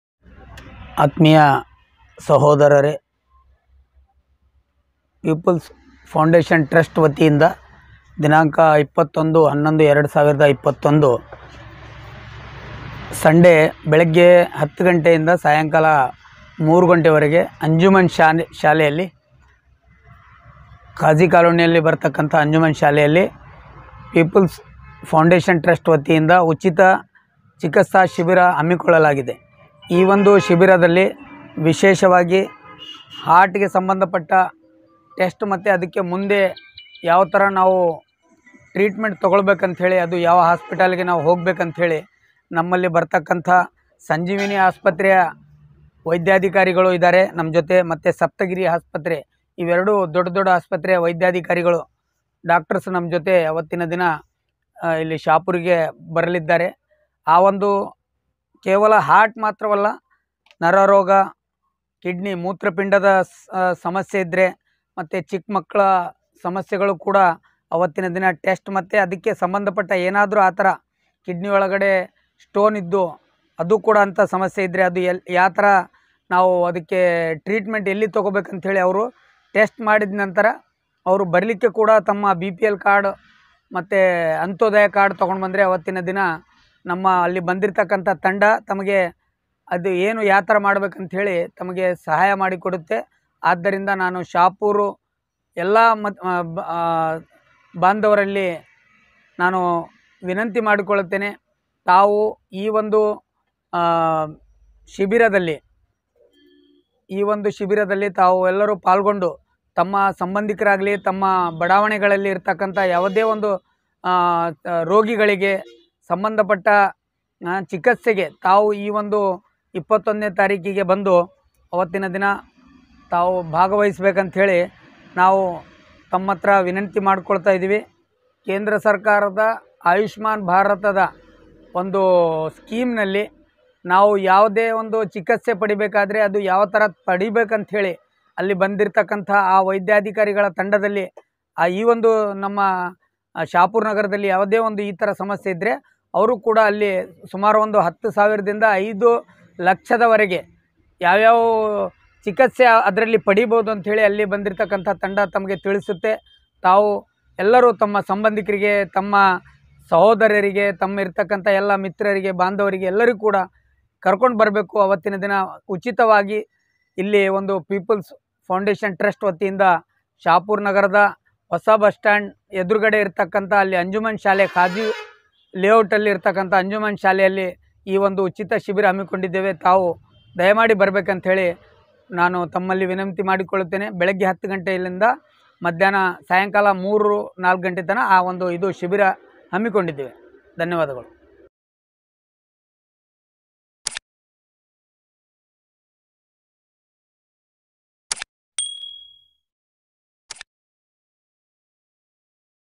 free Megha health Check Up Camp & Treatment || on 21th November 2021 Sunday
SYED ISHAQ HUSSAIN KHALID SDPI Karnataka State Treasurer

Appeal To Join The Free Megha Health Check Up Camp  & Treatment

People's Foundation TRUST (R) Shahapur

21th November Sunday
10am to 3pm

At ANJUMAN SCHOOL
QUAZI COLONY SHAHAPUR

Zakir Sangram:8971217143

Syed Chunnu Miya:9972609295